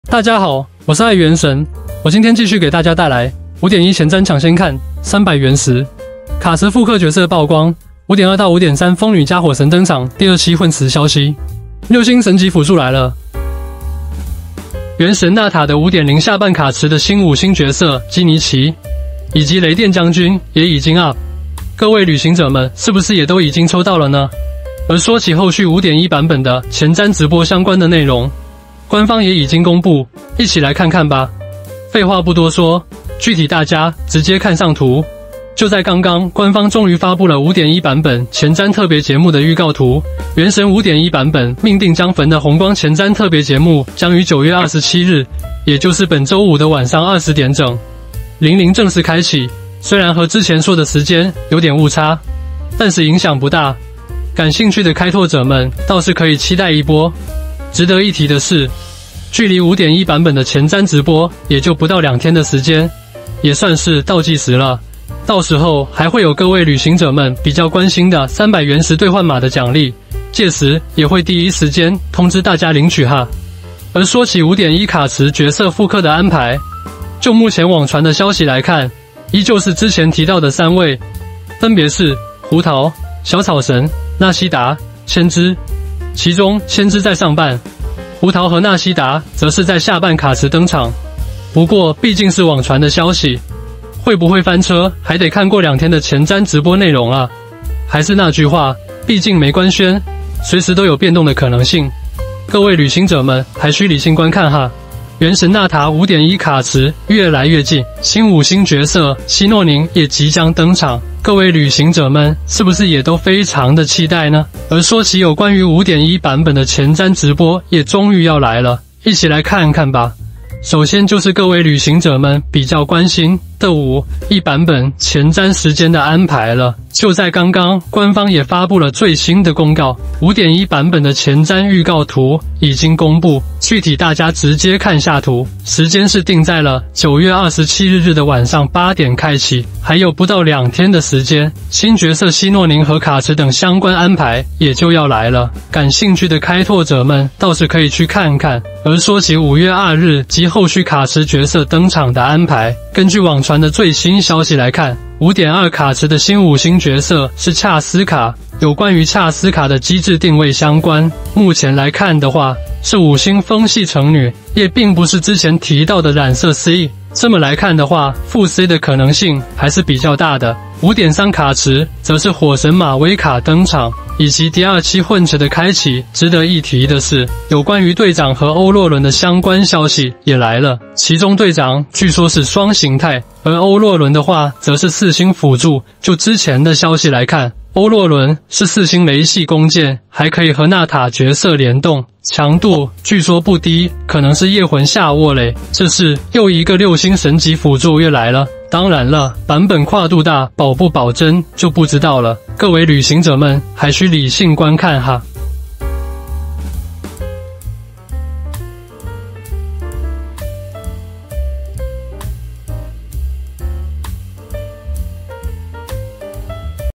5.1前瞻搶先看，300原石！卡池復刻角色曝光，5.2-5.3風女+火神登場，第二期混池消息！六星神級輔助來了！
5.1 preview, 300 original stones! The card pool reprint characters are revealed, 5.2-5.3 Janna + Vulcan debut, the second mixed pool news! Six-star god-level support is here!